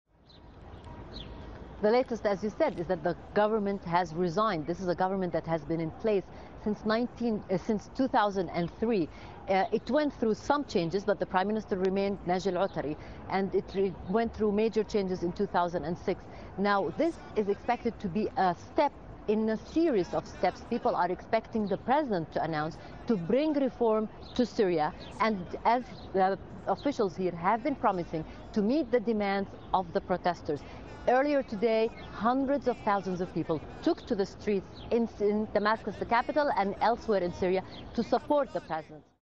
Syria update: Cabinet resigns amid unrest
The Syrian government has resigned, an offer accepted by President Bashar al-Assad.

We are also hearing Assad has appointed Naji Otari - the head of the now defunct government, as the new caretaker prime minister.

The president is expected to address the public in the coming hours.

Al Jazeera's Rula Amin, reporting from the capital Damascus, has the latest.